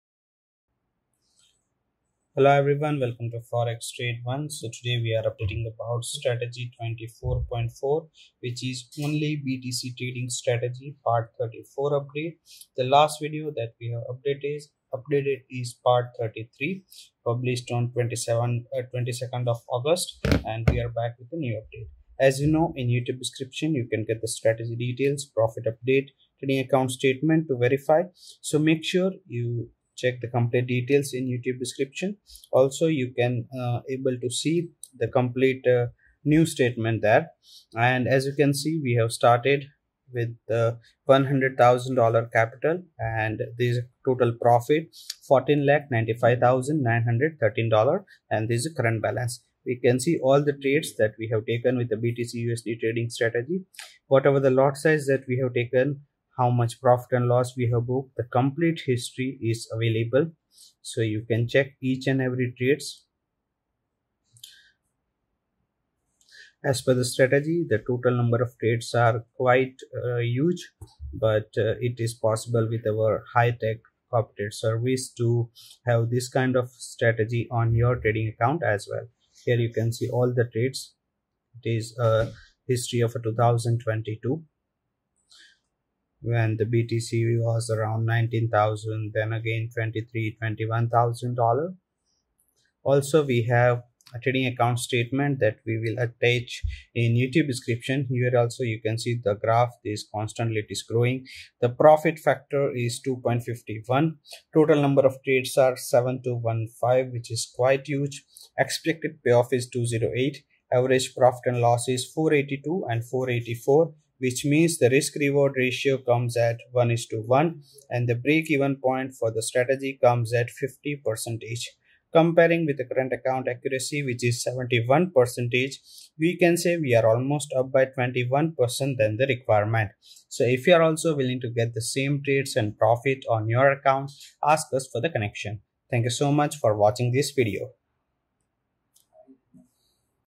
Strategy 24.4 Only BTC Trading with 1502% ROI in 17 Months Part 34
00:10 Introduction
00:30 Verify Previous Update
00:35 Trading Account History
01:35 Trading Account Statement